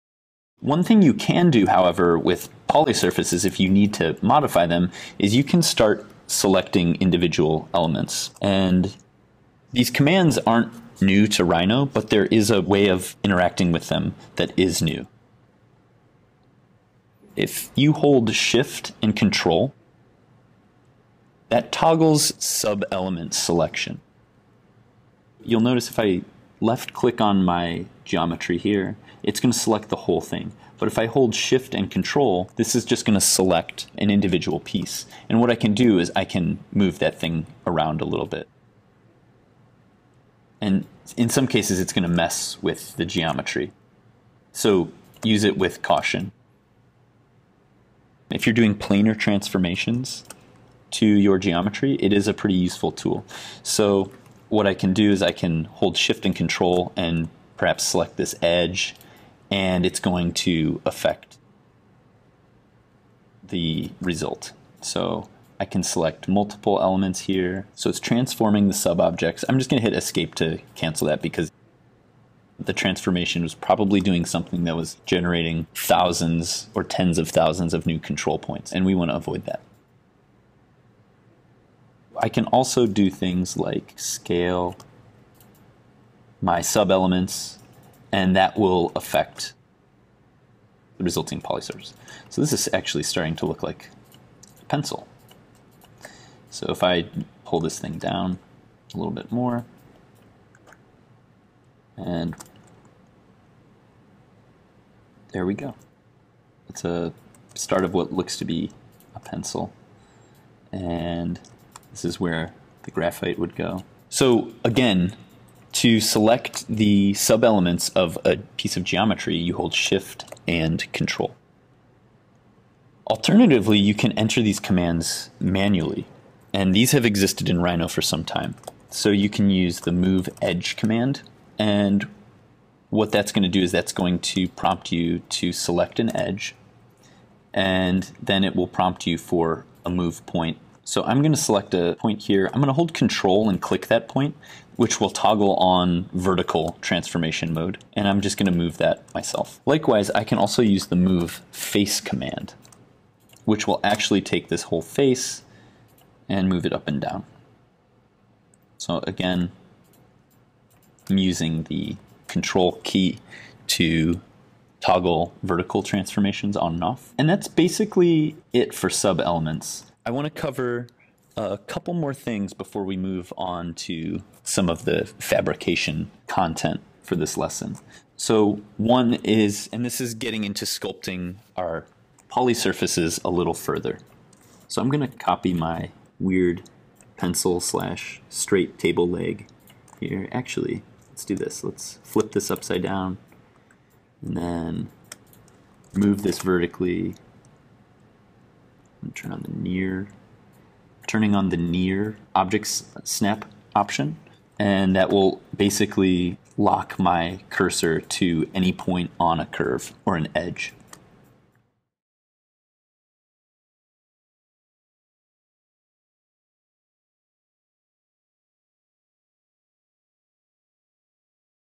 SubElement Select - Architectural Prototyping with Rhino 5 and Grasshopper | Black Spectacles
One thing you can do however, with polysurfaces if you need to modify them, is you can start selecting individual elements and these commands are not new to Rhino but there is a way of interacting with them that is new.

If you hold the "shift" and "control", that toggles sub-element selection. You'll notice if I left click on my geometry here, it's going to select the whole thing. But if I hold shift and control, this is just going to select an individual piece.

And what I can do is I can move that thing around a little bit. And in some cases it's going to mess with the geometry, so use it with caution. If you're doing planar transformations to your geometry, it is a pretty useful tool.

So what I can do is I can hold shift and control and perhaps select this edge and it's going to affect the result. So I can select multiple elements here, so it's transforming sub-objects, I'm just going to hit "escape" to cancel that because the transformation is probably doing something that was generating thousands or tens of thousands of new control points and we want to avoid that.

I can also do things like scale my sub-elements and that will affect the resulting polysurfaces. This is actually starting to look like a pencil. So if I pull this thing down a little bit more, and there we go. It started what looks to be a pencil. And this is where the graphite would go.

Again, to select the sub-elements of a piece of geometry, you hold shift and control. Alternatively you can enter these commands manually and these have existed in Rhino for some time. So you can use the "move edge" command and what that's going to do is that's going to prompt you to select an edge and then it will prompt you for a move point.

So I'm going to select a point here. I'm going to hold "control" and click that point which will toggle on vertical transformation mode and I'm just going to move that myself.

Likewise I can also use the "MoveFace" command which will actually take this whole face and move it up and down. So again using the "control" key to toggle vertical transformations on and off, and that's basically it for sub-elements.

I want to cover a couple more things before we move on to some of the fabrication content for this lesson. So one is, and this is getting into sculpting our polysurfaces a little further. So I'm going to copy my weird pencil/straight table leg here. Actually let's do this. Let's flip this upside down and then move this vertically, and turn on the near, turning on the near objects snap option and that will basically lock my cursor to any point on a curve or an edge.